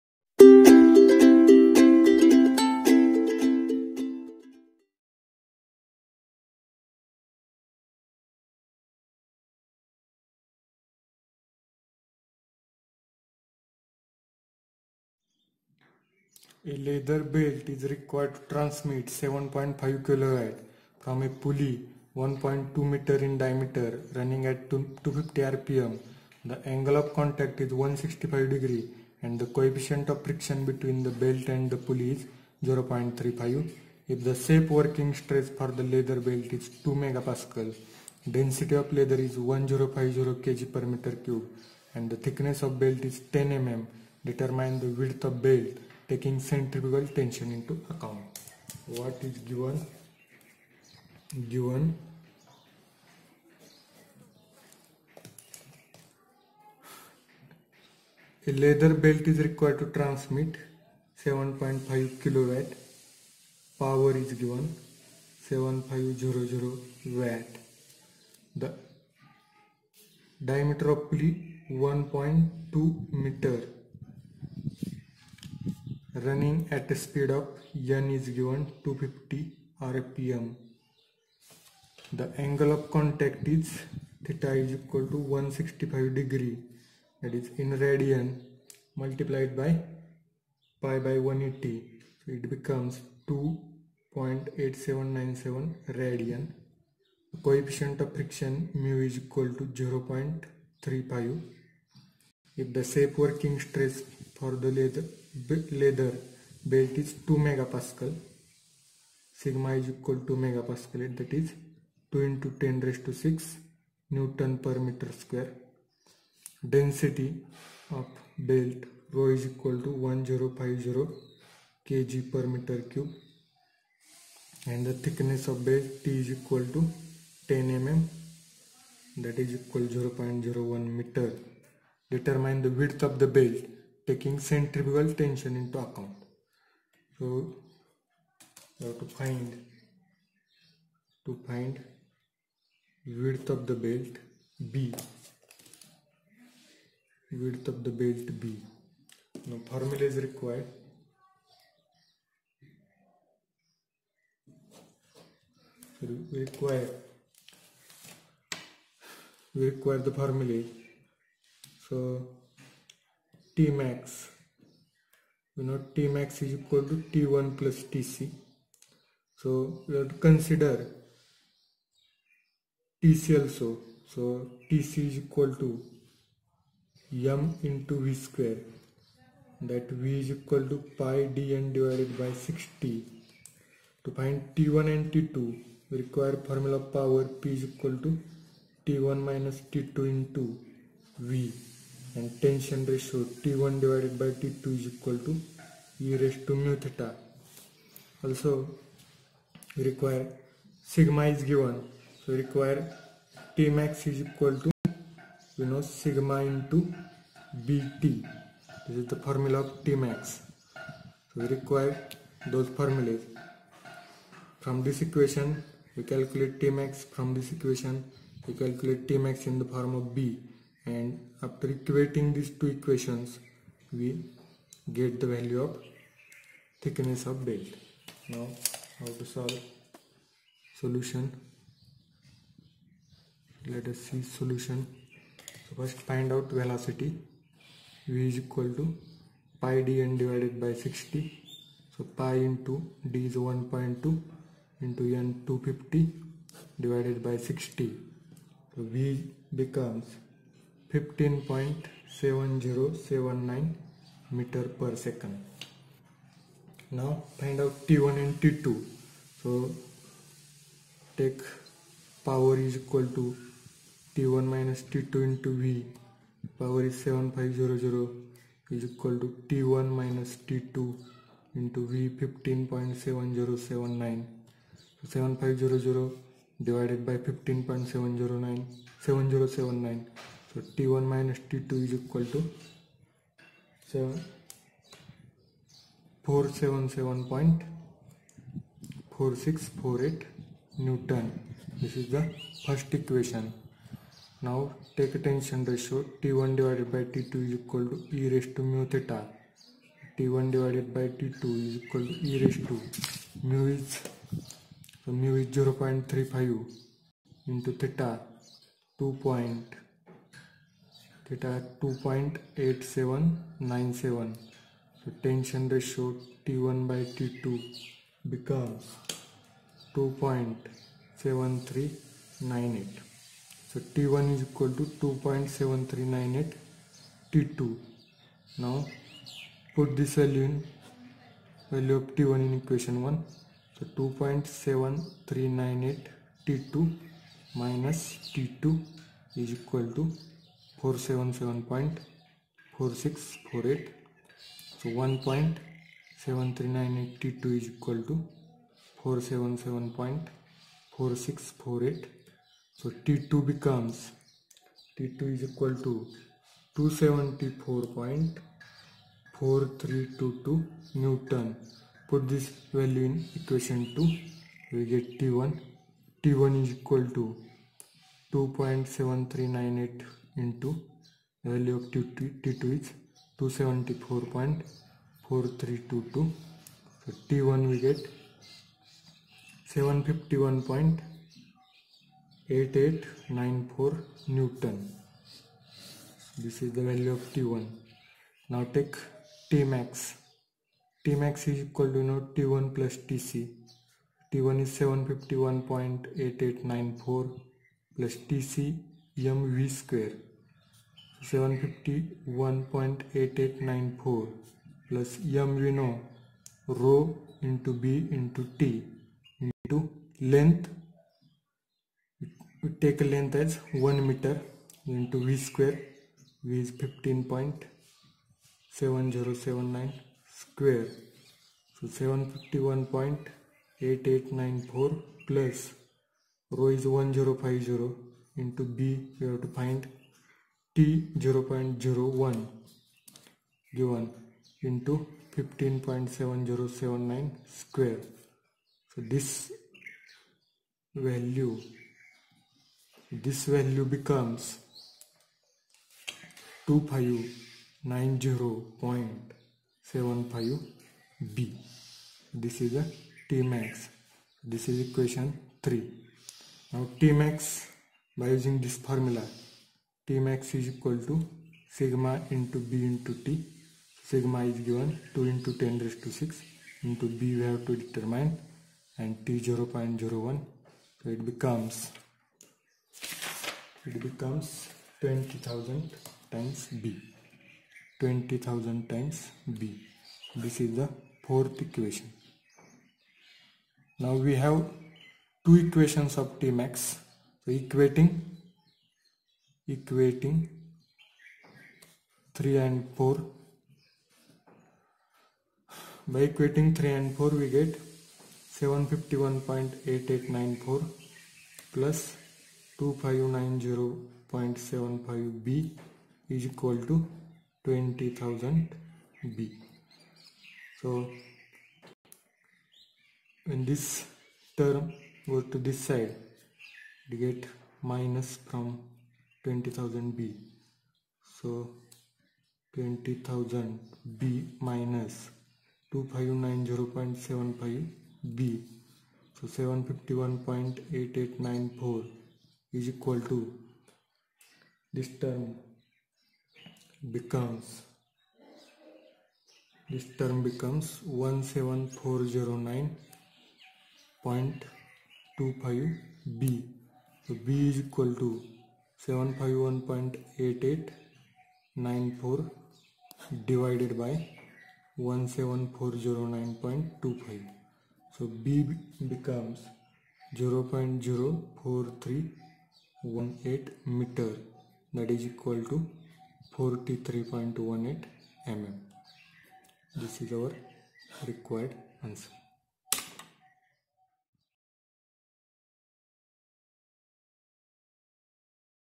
Belt Drive (Problem - 02) ।। How to Find Width of Belt
In this video I solved problem of belt drive in which width of belt is to be find.